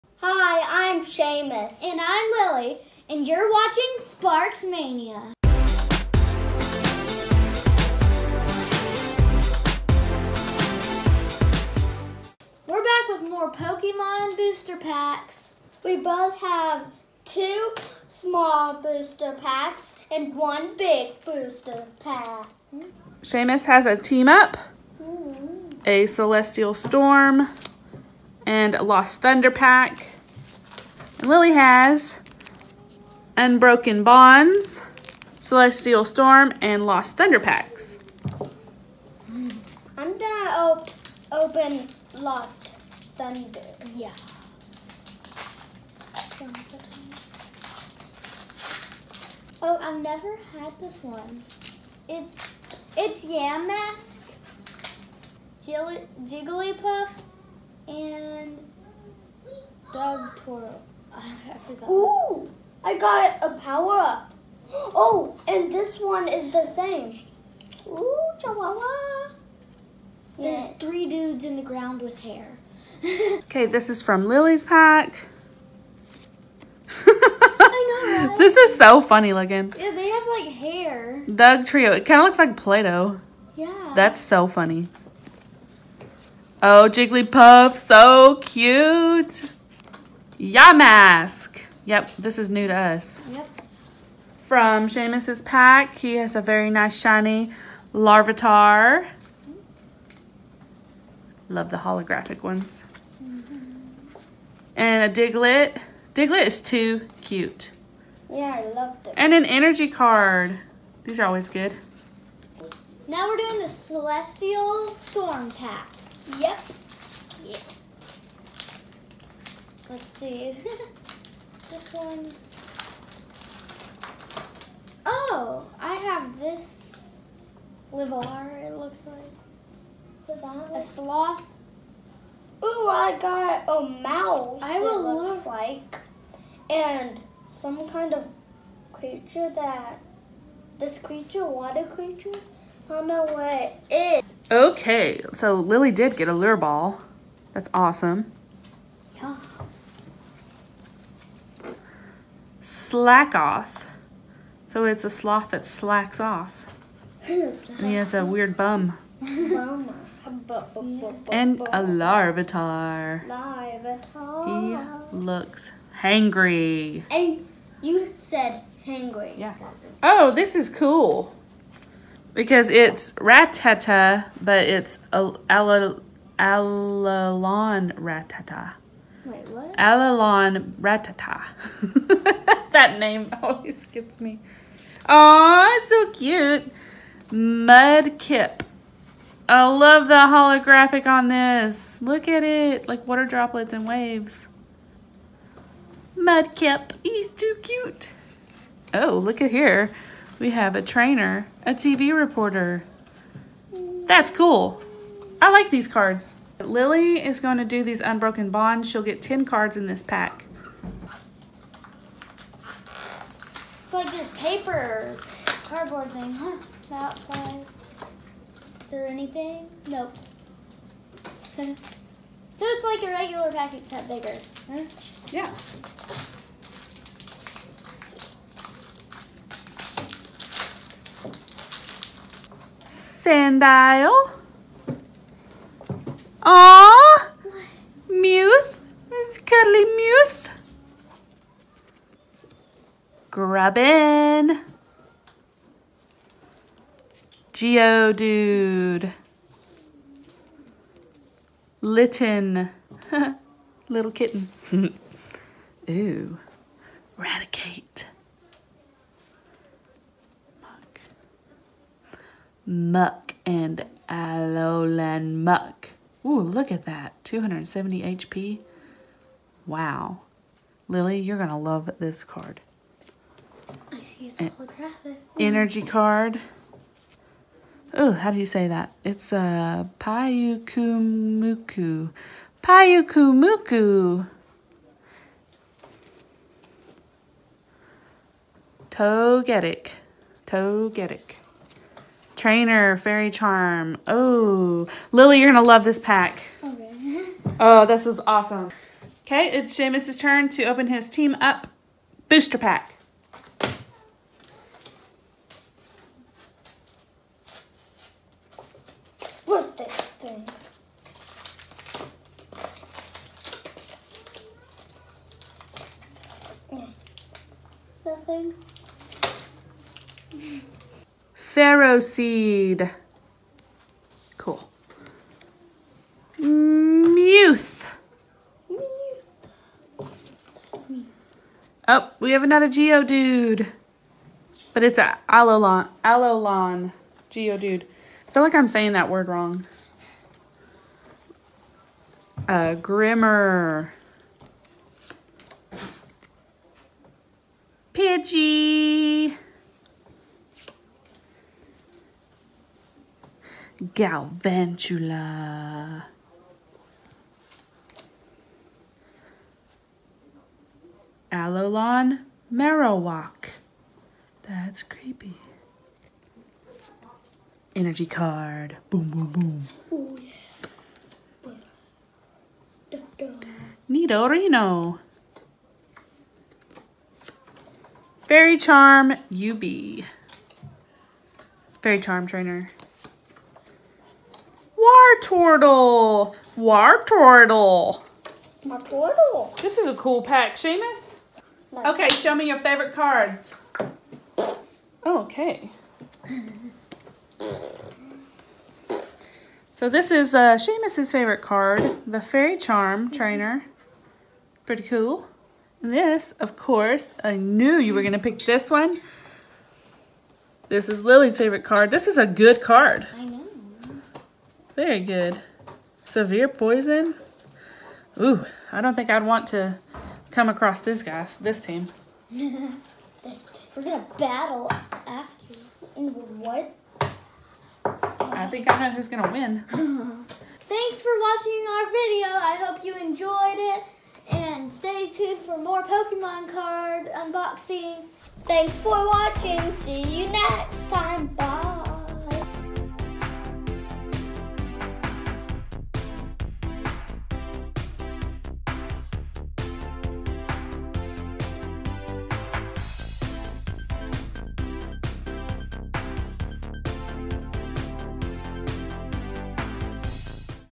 Opening Sun & Moon Pokemon Packs
This was our first time finding Alola Pokemon!! These packs were awesome!